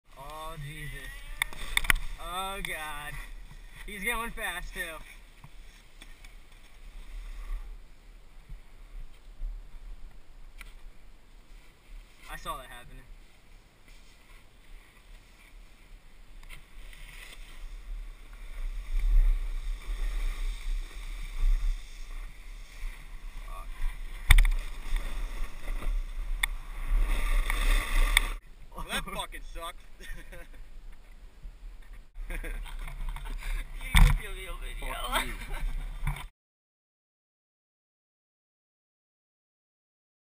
snowboard box fail GoPro
watching my brother falling on a rail and hitting his face on ice